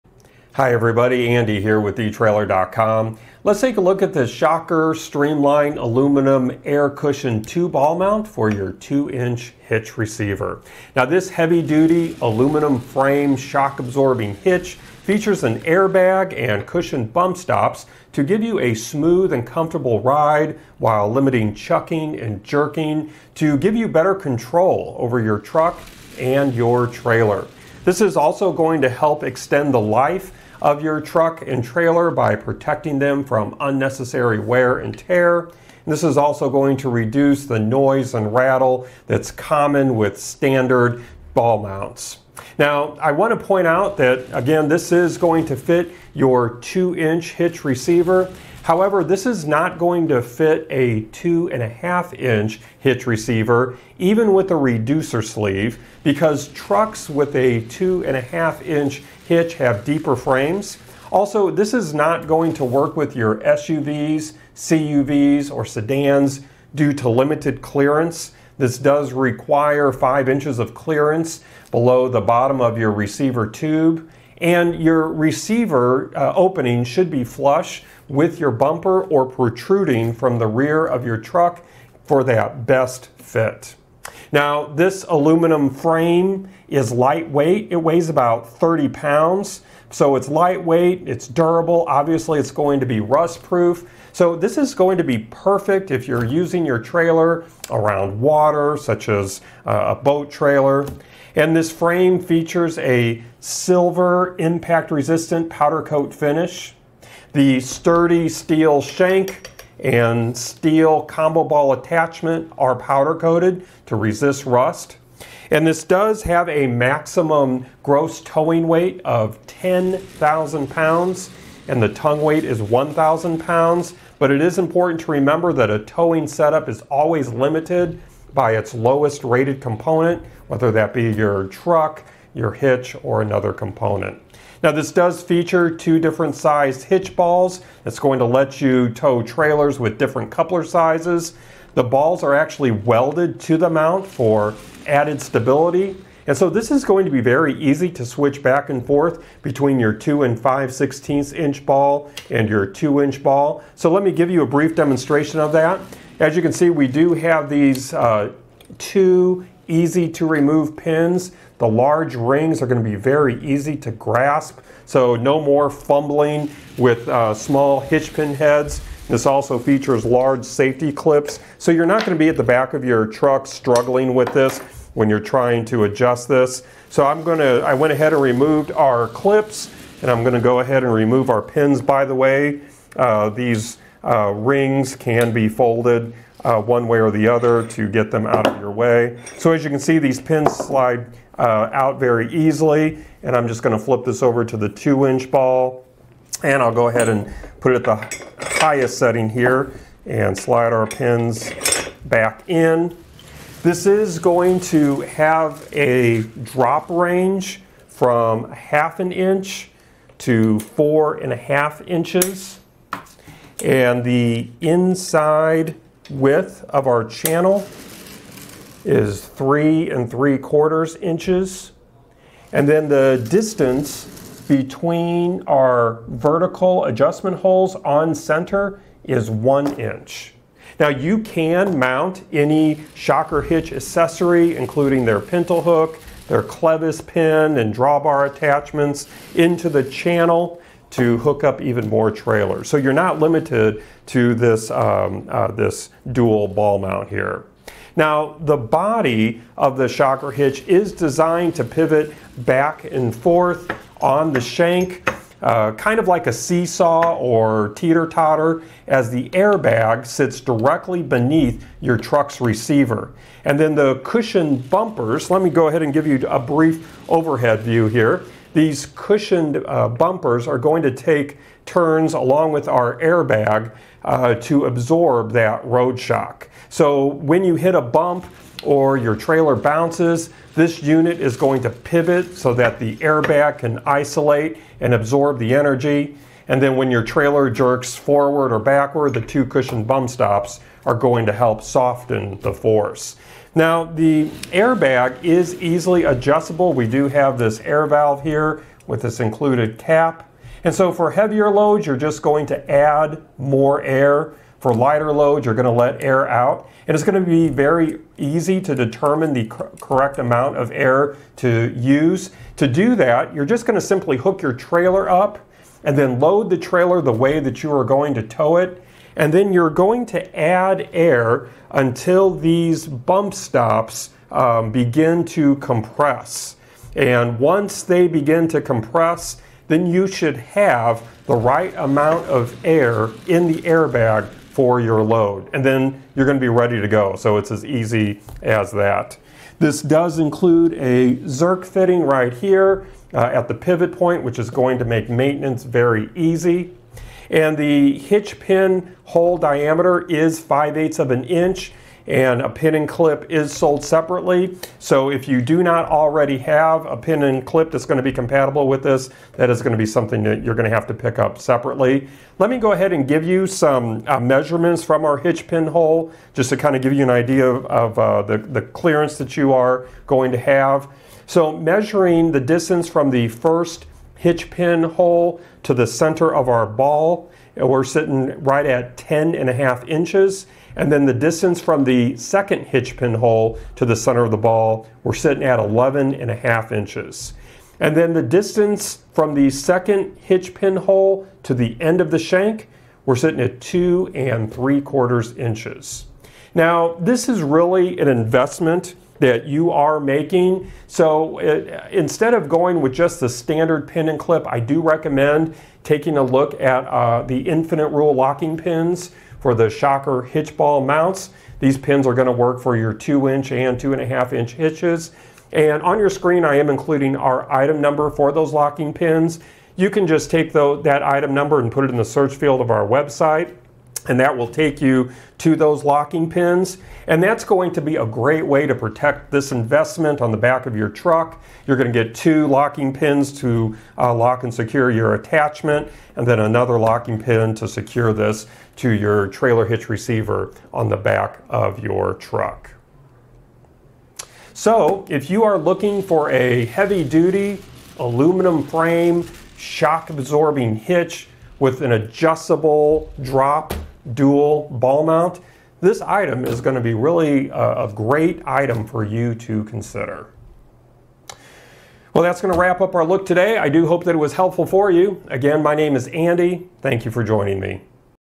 etrailer | Hands-On with the Shocker Streamline Aluminum Air Cushioned 2-Ball Mount
Click for more info and reviews of this Shocker Hitch Trailer Hitch Ball Mount:

Hi, everybody, Andy here with etrailer.com.  Let's take a look at the Shocker streamlined aluminum  air cushion two-ball mount for your two-inch hitch receiver.  Now, this heavy duty aluminum frame shock absorbing hitch  features an airbag and cushion bump stops  to give you a smooth and comfortable ride  while limiting chucking and jerking  to give you better control over your truck and your trailer.  This is also going to help extend the life  of your truck and trailer by protecting them  from unnecessary wear and tear.  And this is also going to reduce the noise  and rattle that's common with standard ball mounts.  Now, I wanna point out that, again, this is going  to fit your two-inch hitch receiver.

However, this is not going to fit a 2.5-inch  hitch receiver even with a reducer sleeve  because trucks with a 2.5-inch hitch have deeper frames.  Also, this is not going to work with your SUVs, CUVs,  or sedans due to limited clearance.  This does require five inches of clearance  below the bottom of your receiver tube,  and your receiver opening should be flush with your bumper  or protruding from the rear of your truck for that best fit.  Now, this aluminum frame is lightweight,  it weighs about 30 pounds.  So it's lightweight, it's durable.

Obviously, it's going to be rust proof.  So this is going to be perfect if you're using your trailer  around water such as a boat trailer.  And this frame features a silver  impact-resistant powder coat finish.  The sturdy steel shank and steel combo ball attachment  are powder coated to resist rust.  And this does have a maximum gross towing weight  of 10,000 pounds and the tongue weight is 1000 pounds.

But it is important to remember  that a towing setup is always limited  by its lowest rated component, whether that be your truck,  your hitch, or another component.  Now, this does feature two different-sized hitch balls.  That's going to let you tow trailers  with different coupler sizes.  The balls are actually welded to the mount  for added stability and so this is going to be very easy  to switch back and forth between your 2 5/16-inch ball  and your two-inch ball.  So let me give you a brief demonstration of that.

As you can see, we do have these two easy-to-remove pins.  The large rings are gonna be very easy to grasp,  so no more fumbling with small hitch pin heads.  This also features large safety clips.  So you're not gonna be at the back of your truck,  struggling with this when you're trying to adjust this.  So I went ahead and removed our clips  and I'm gonna go ahead and remove our pins.  By the way, these rings can be folded  one way or the other to get them out of your way.  So as you can see, these pins slide out very easily.  And I'm just gonna flip this over to the two-inch ball  and I'll go ahead and put it at the highest setting here  and slide our pins back in.  This is going to have a drop range  from 1/2 inch to 4.5 inches,  and the inside width of our channel is 3 3/4 inches.  And then the distance between our vertical adjustment holes  on center is one inch.  Now, you can mount any Shocker Hitch accessory  including their pintle hook, their clevis pin,  and drawbar attachments into the channel  to hook up even more trailers.  So you're not limited to this dual ball mount here.  Now, the body of the Shocker Hitch is designed  to pivot back and forth on the shank,  kind of like a seesaw or teeter totter  as the airbag sits directly beneath your truck's receiver.  And then the cushion bumpers, lemme go ahead  and give you a brief overhead view here.  These cushioned bumpers are going to take turns  along with our airbag to absorb that road shock.  So when you hit a bump or your trailer bounces,  this unit is going to pivot  so that the airbag can isolate and absorb the energy.  And then when your trailer jerks forward or backward,  the two cushion bump stops  are going to help soften the force.  Now, the airbag is easily adjustable.  We do have this air valve here with this included cap.  And so for heavier loads, you're just going to add more air.  For lighter loads, you're gonna let air out.  It is gonna be very easy to determine  the correct amount of air to use.  To do that, you're just gonna simply hook your trailer up  and then load the trailer the way  that you are going to tow it.  And then you're going to add air  until these bump stops begin to compress.  And once they begin to compress,